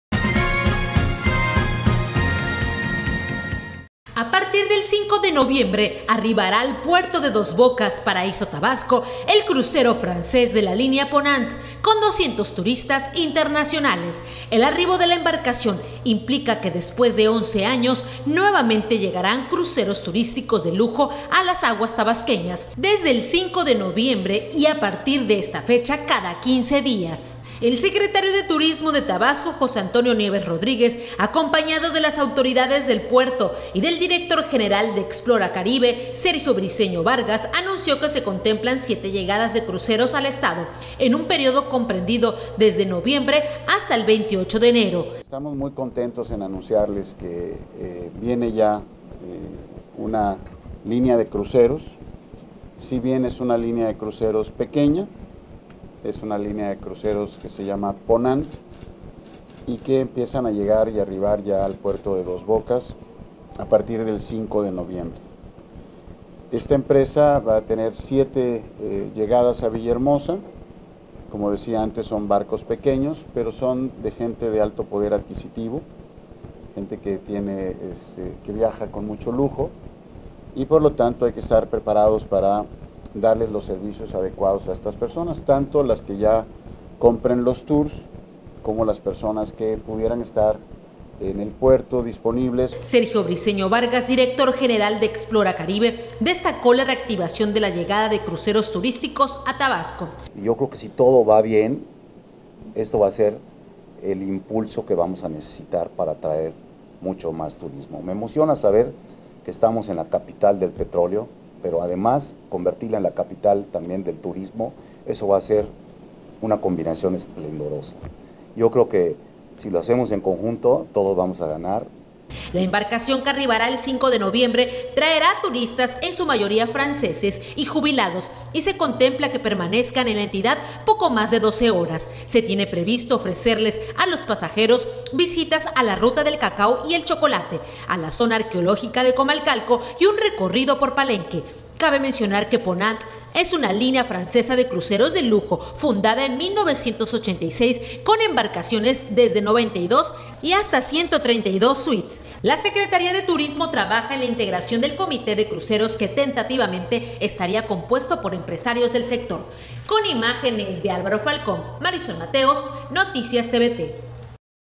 Después de 11 años, se reactiva la llegada de cruceros turísticos a Dos Bocas
NoticiasTVT Después de 11 años que no atracaban cruceros en Dos Bocas, el 5 de noviembre arribará al puerto paraiseño el barco francés "Ponant" con 200 turistas, anuncia el secretario de Turismo.